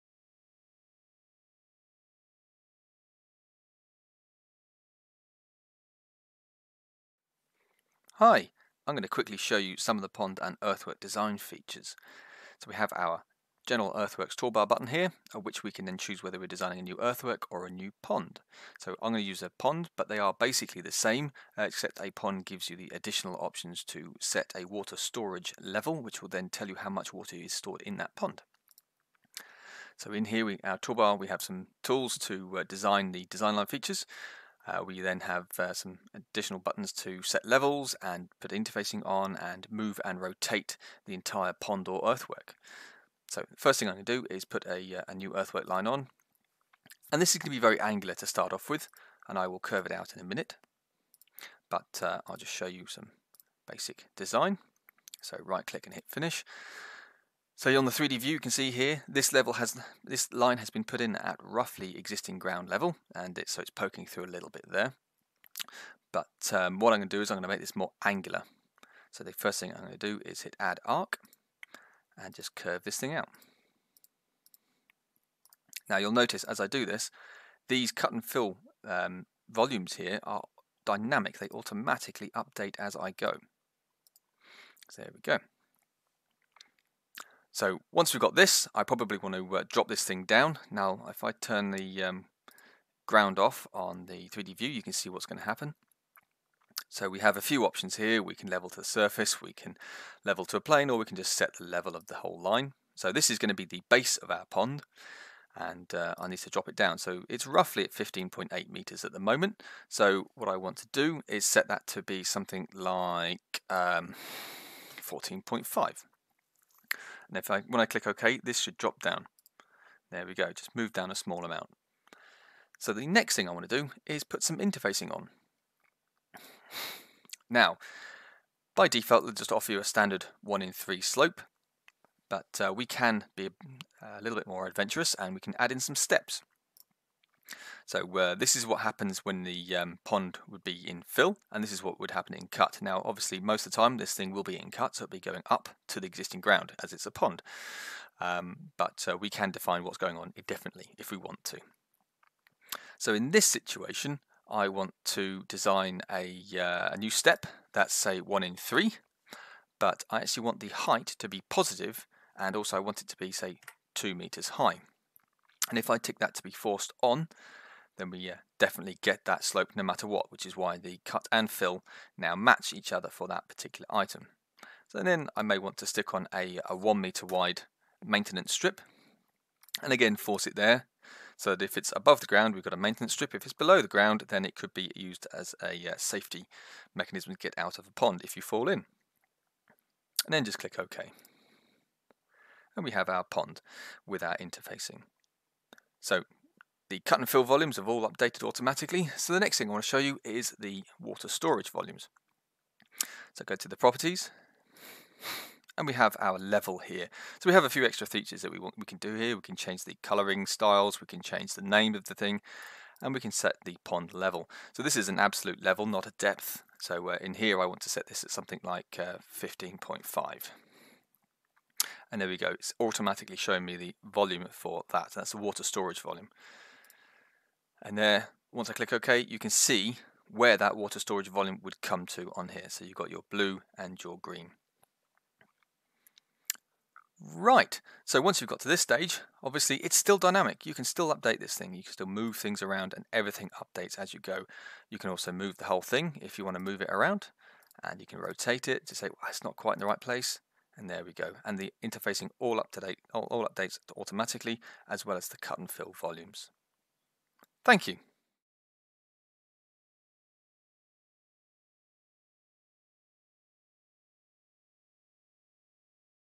Pond Design Features
A brief overview of pond and earthwork design features in Site3D.